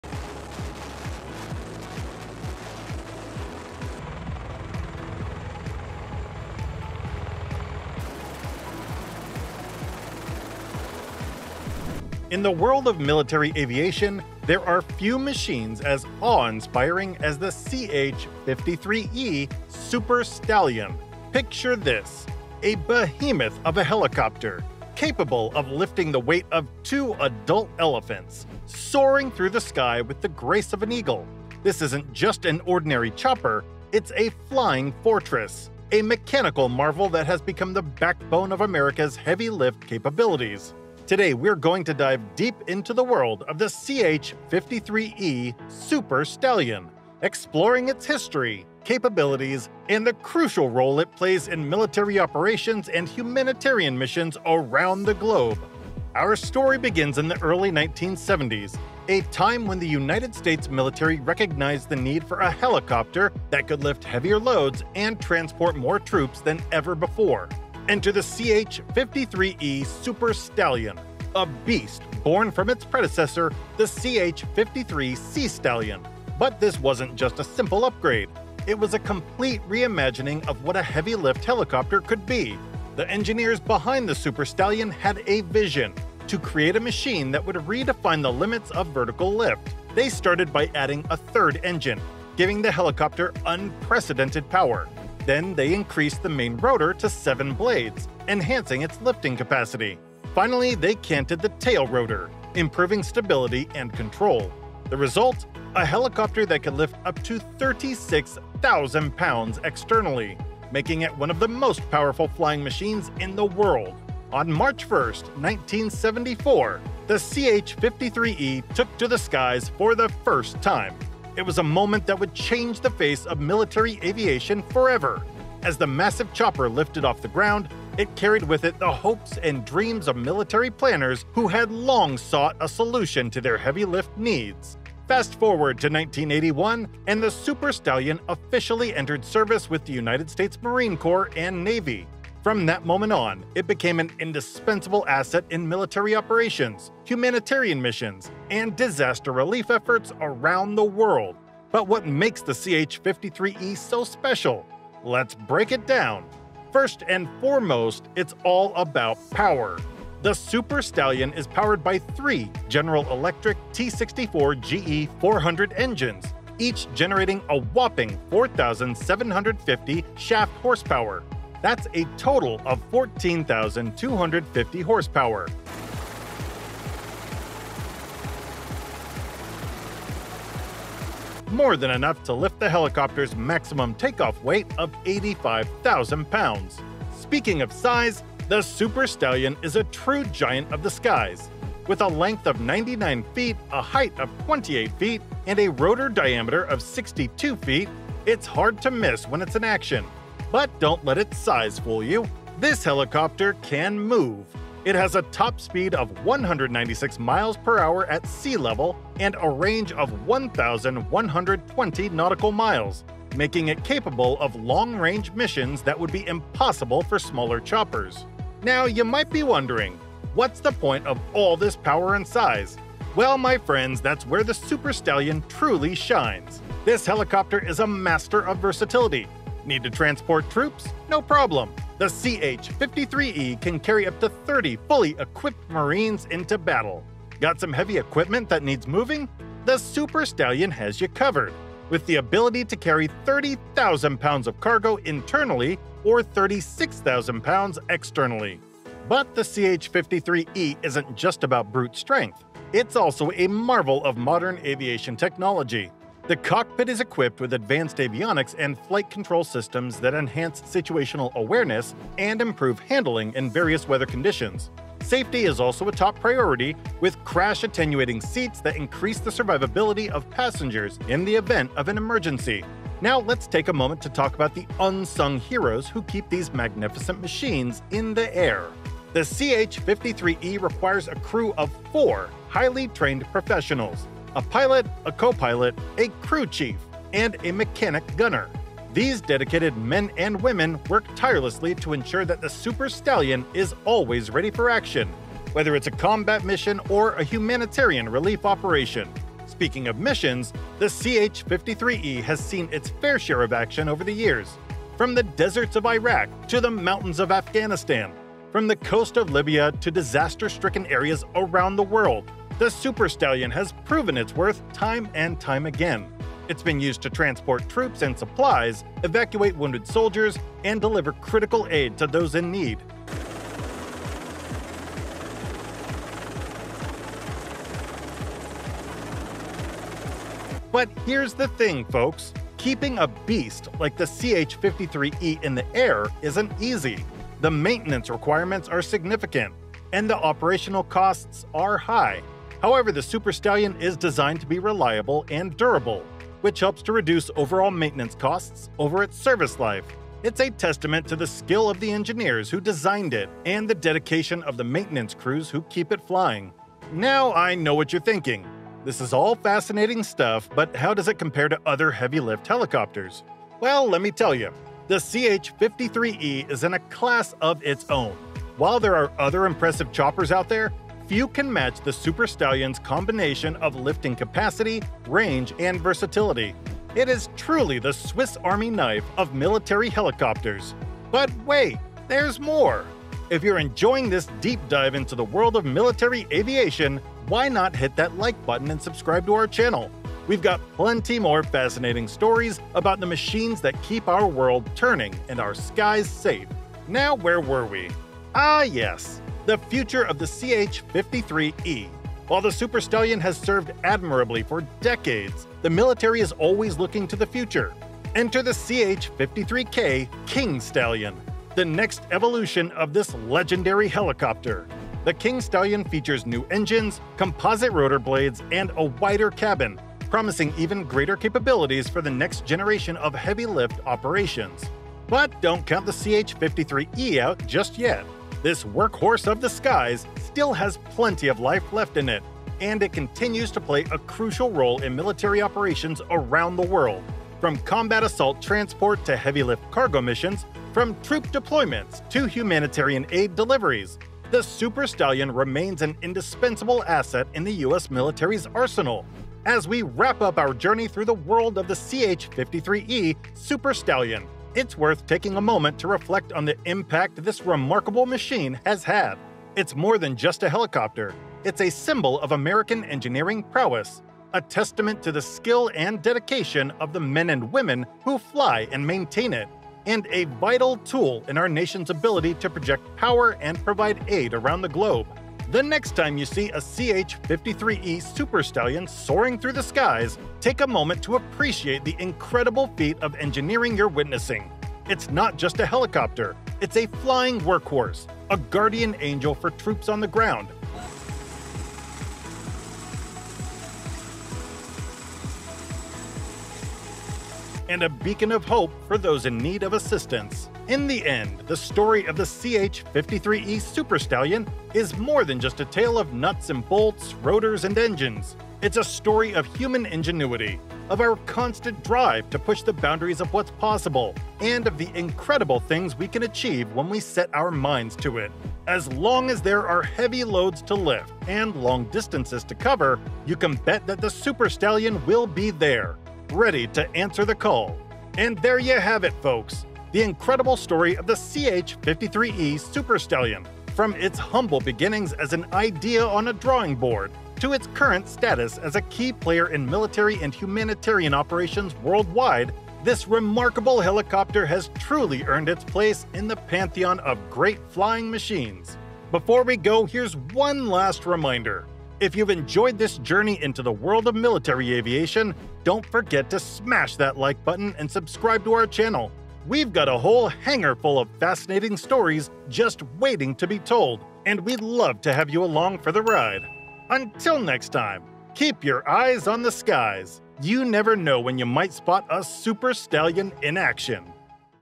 The MONSTER CH-53E: America's Sky Titan That Enemies Fear to Face!
Discover the incredible story of the CH-53E Super Stallion, America's most powerful heavy-lift helicopter. From its origins in the 1970s to its vital role in military operations and humanitarian missions, the CH-53E is a marvel of aviation engineering. Learn about its unmatched lifting capacity, advanced technology, and the dedicated crew who keep it flying. Explore how this flying workhorse continues to serve the U.S. military and what the future holds with the CH-53K King Stallion. Join us as we dive deep into the world of this extraordinary machine.